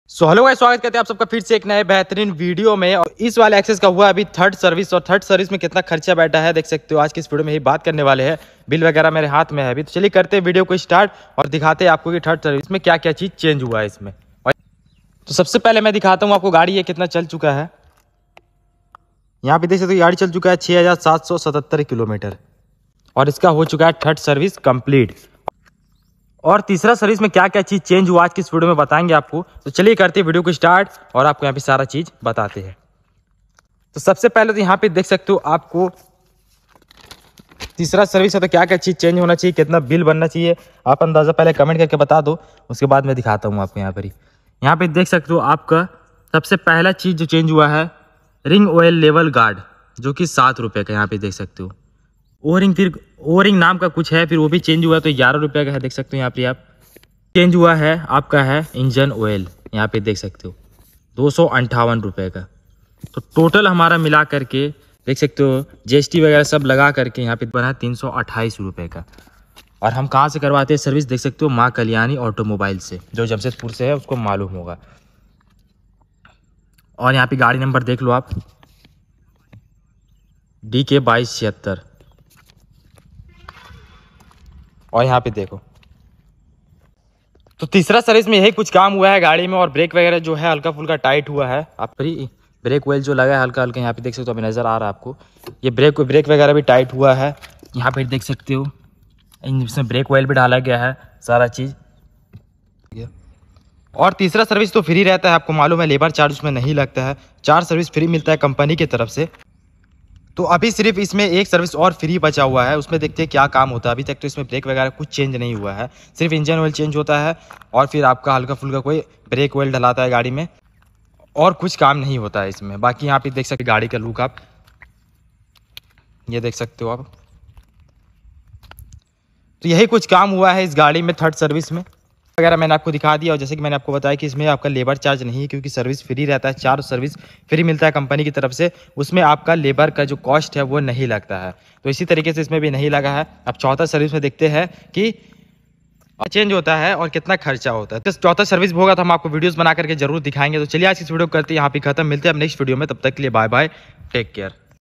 3rd Service Me इतना खर्च होगा सोचा नहीं था 🤔 | Access 125 | Suzuki Access 125 Review 🔥
3rd Service Me इतना खर्च होगा सोचा नहीं था 🤔 | Suzuki Access 125 | Suzuki Access 125 Review 🔥

Your Queries Sloved